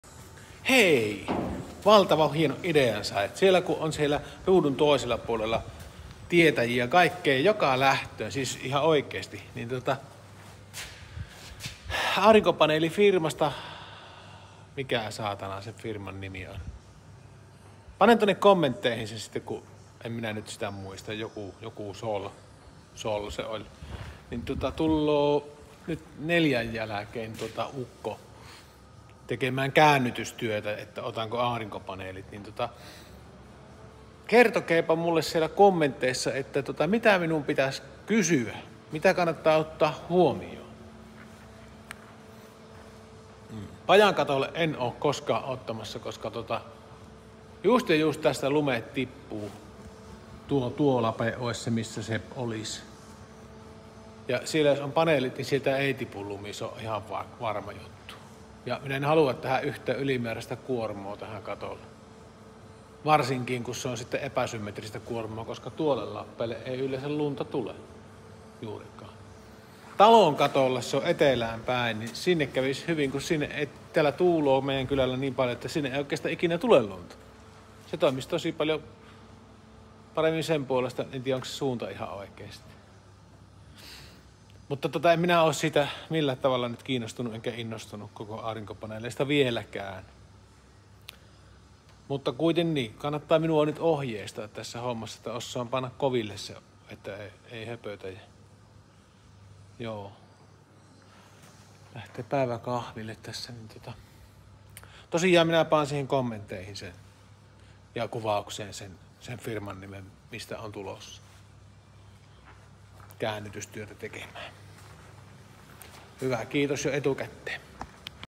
(Edit: peruin tapaamisen, kiitos) Aurinkopaneelin tyrkyttäjä tulloo,, vinkkejä ja neuvoja kaipaan.
Kotisol oli se firma